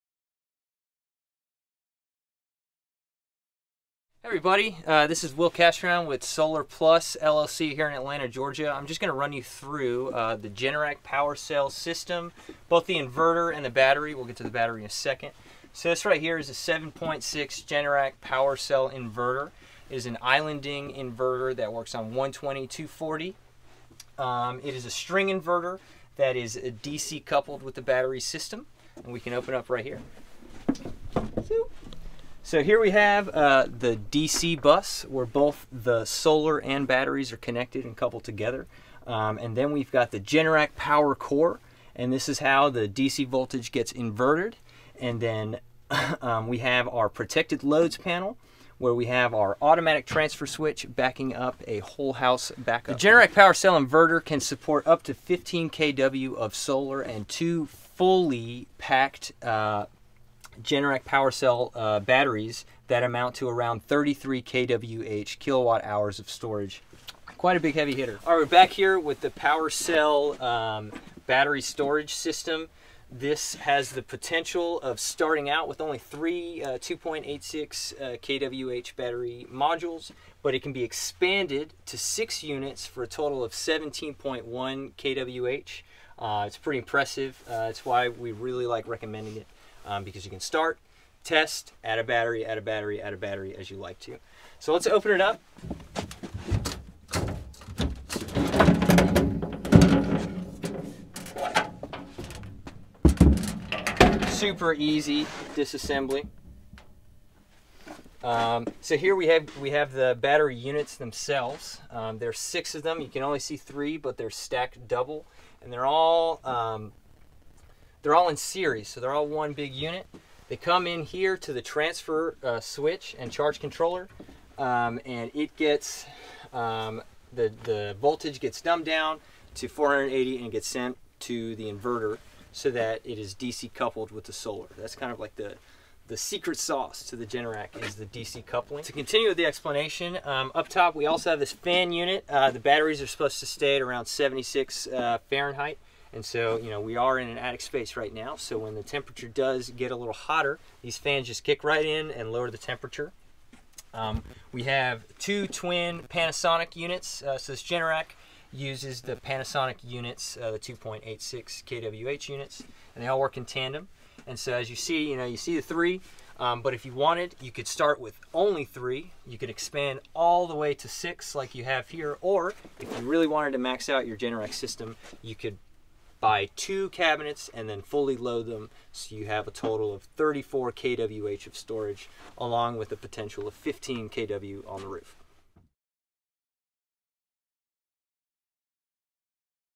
Solar Plus - Generac PWRcell Explained
Ever wonder how a Generac PWRcell battery storage system works? Senior project manager William Castrillon stops by to explain.